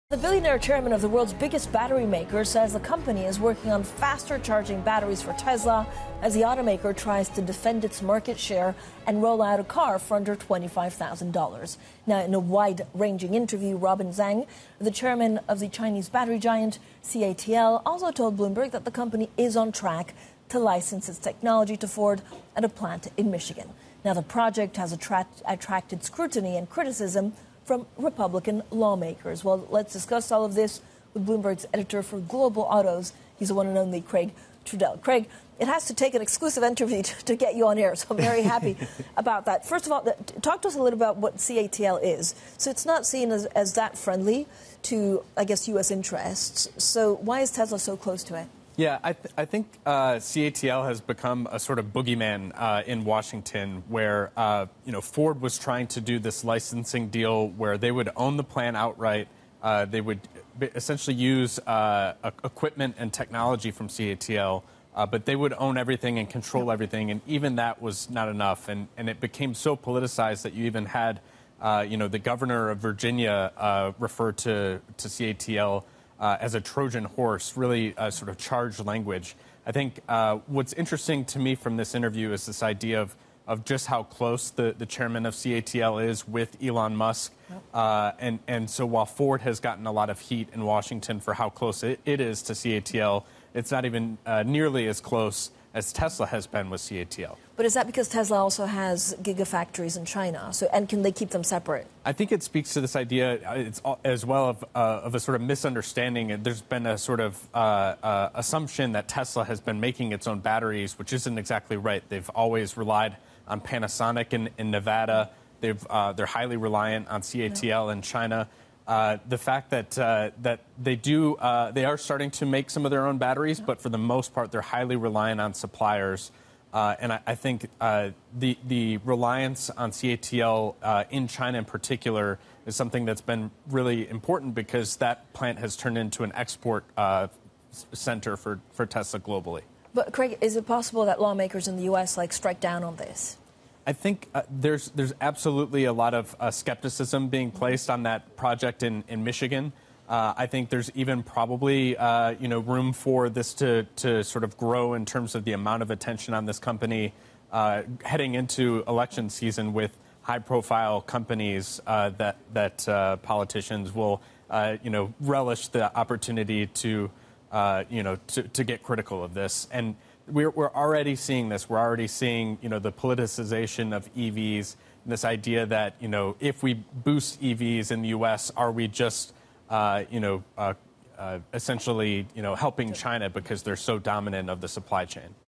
Electric Cars: CATL Is Working on Fast-Charging Cells for Tesla
CATL is working on faster charging batteries for Tesla as Elon Musk’s automaker seeks to defend its market share and roll out an electric car for under $25,000, Robin Zeng, the billionaire chairman of the world’s biggest battery maker, said in a wide-ranging interview with Bloomberg News in Hong Kong on Monday. Bloomberg's Craig Trudell reports.